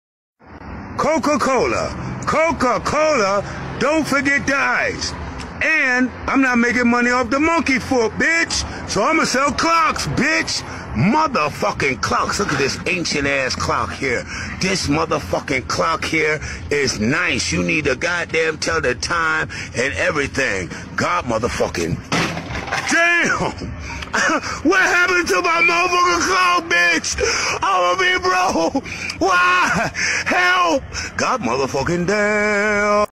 Rebel's Clocks Selling| Evade Meme
Rebel is gonna get rich after selling clocks or maybe not?!
I didn't really want to draw the clock, so I just get some clocks pngs and oh man does it make it funnier
Also Cheeseburger